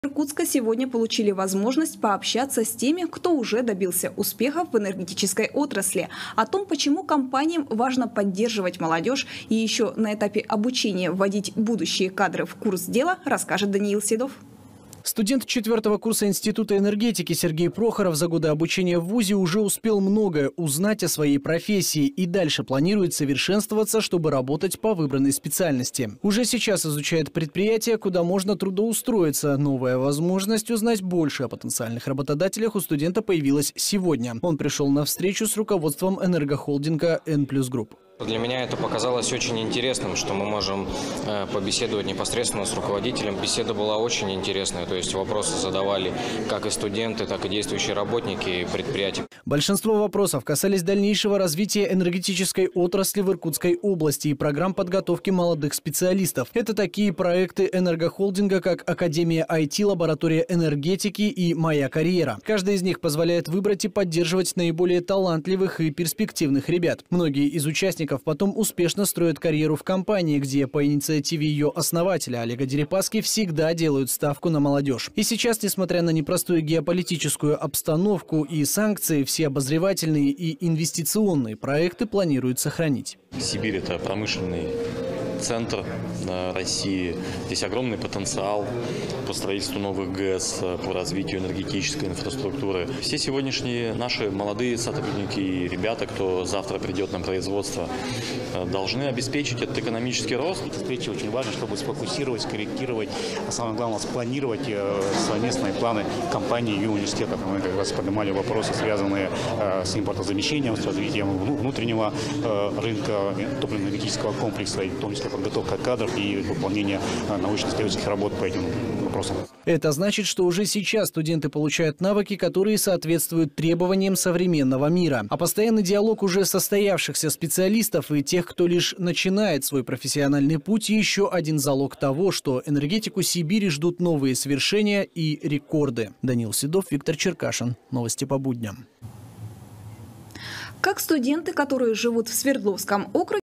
Встреча с руководством холдинга En+ Group
Руководитель энергетического бизнеса En+ Group Михаил Хардиков встретился со студентами ИРНИТУ.

Михаил Хардиков рассказал о перспективах развития отрасли в текущих экономических условиях, обозначил ориентиры развития для нового поколения энергетиков. Он озвучил основные направления текущей стратегии компании и задачи топливно-энергетического комплекса в новых экономических реалиях.

Студентов интересовали вопросы трудоустройства, преимущества и условия работы на предприятиях En+ Group.